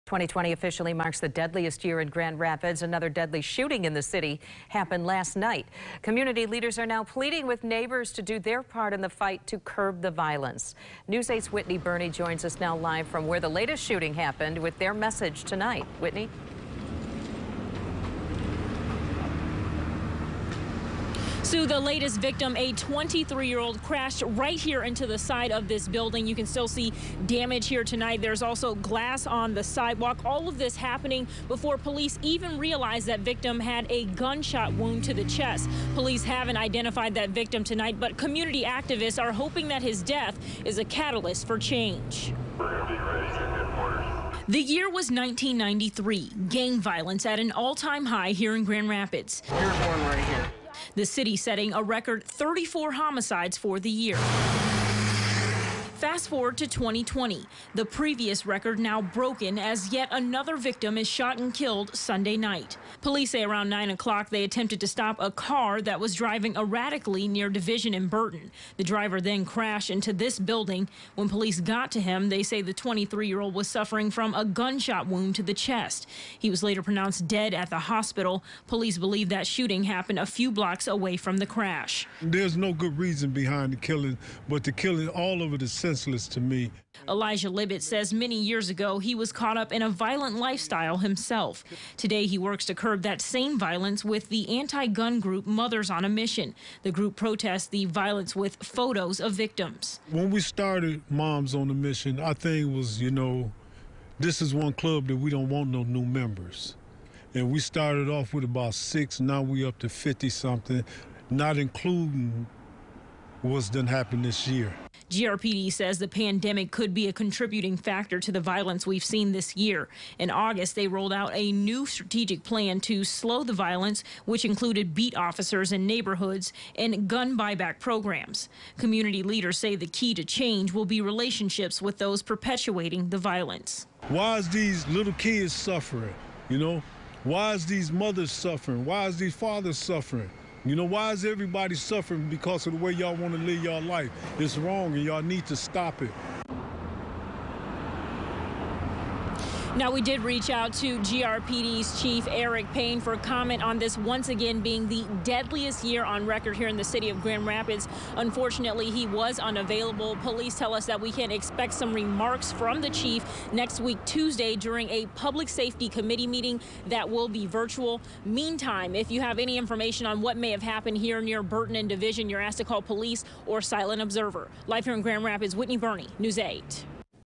‘It needs to stop’: Grand Rapids breaks record for deadliest year
2020 is the deadliest year on record for the city of Grand Rapids. (Dec. 7, 2020)